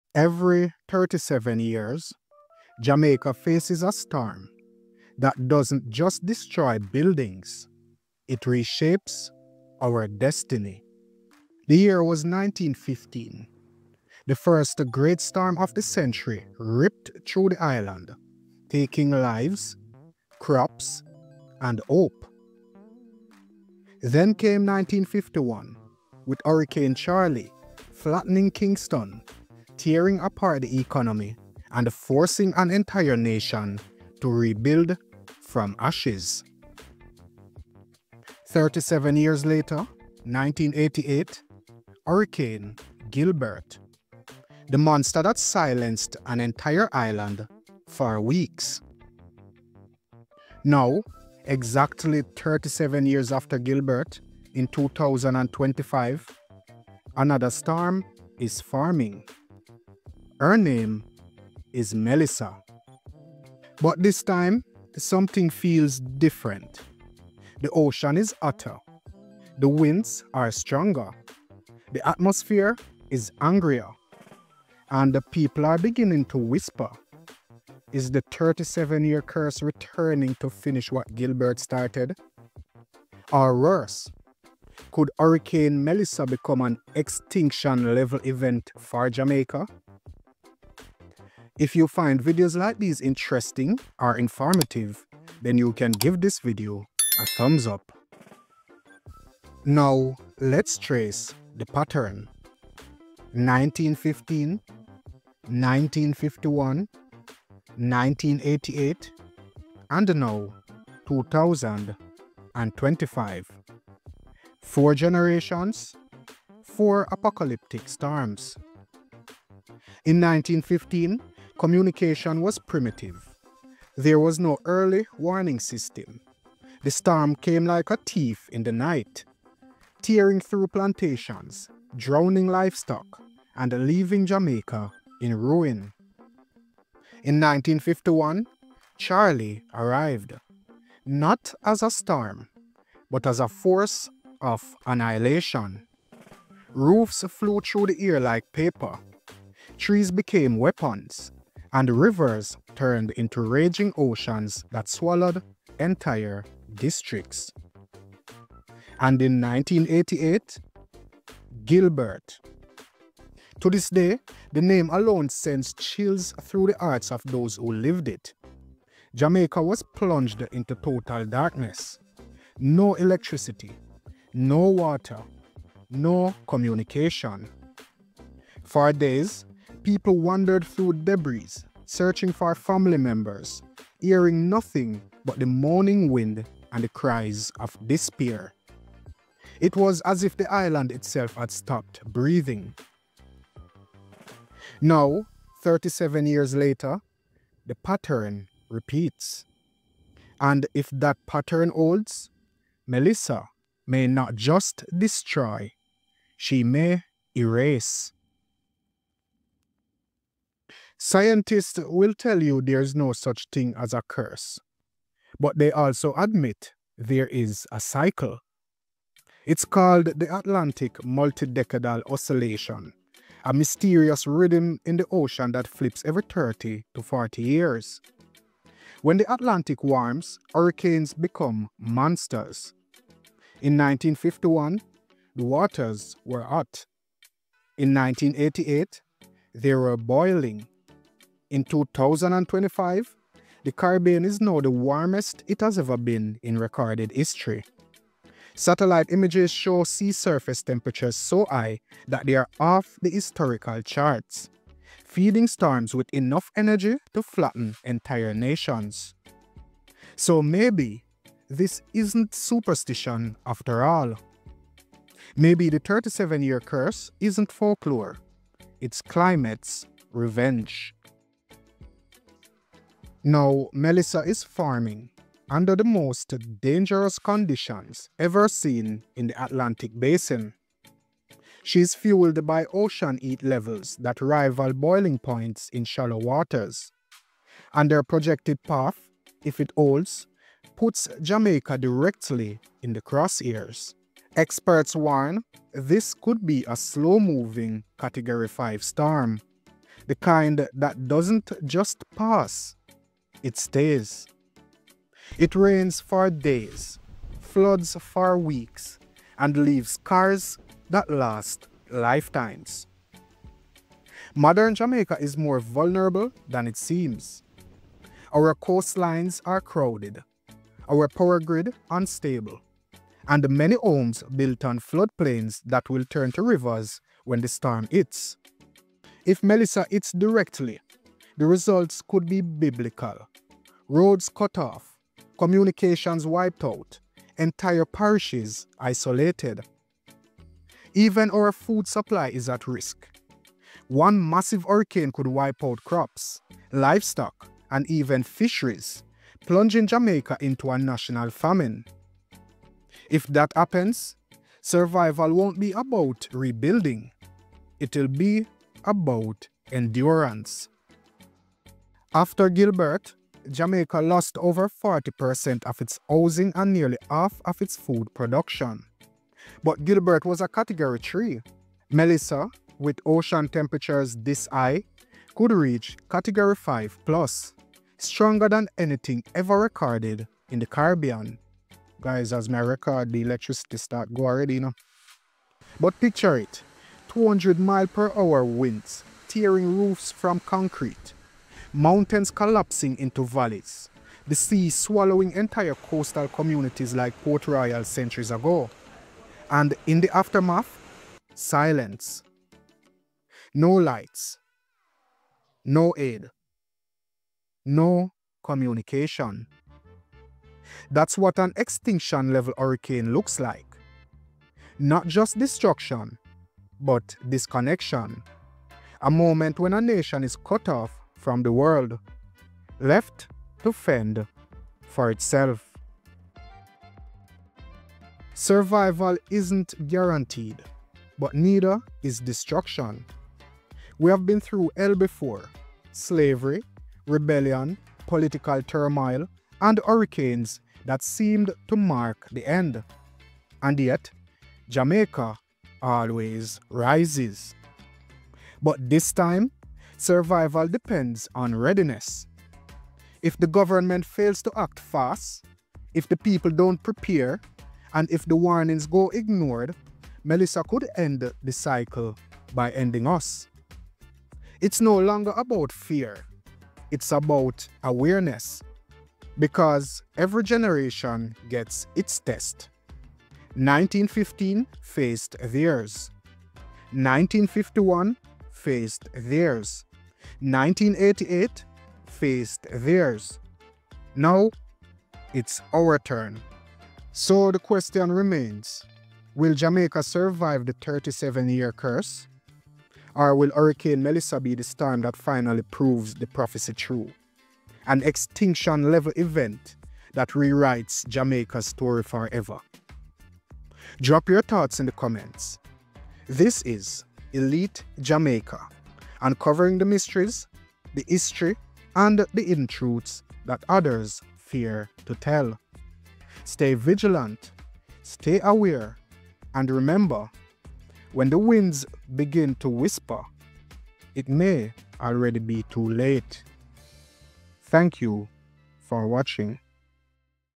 Will Hurricane Melissa Kill Us? — The 37-Year Curse That Haunts Jamaica
Will Hurricane Melissa Be an Extinction-Level Event? — The 37-Year Curse That Haunts Jamaica

🧾 Description:

For over a century, Jamaica has faced a terrifying pattern — every 37 years, a hurricane strikes that changes the course of our history. From the hurricane of 1915 to Charlie in 1951, to Gilbert in 1988 — and now, 2025 — Hurricane Melissa is forming under the most dangerous ocean conditions ever recorded.

Is this just coincidence, or is Jamaica trapped in a 37-year curse written into the Earth’s climate? Could Melissa be the storm that ends it all — an extinction-level event for our island?

Join Elite Jamaica Official Channel as we uncover the chilling history, scientific explanations, and dark prophecies surrounding Jamaica’s hurricane cycle. This is not just weather — it’s warning.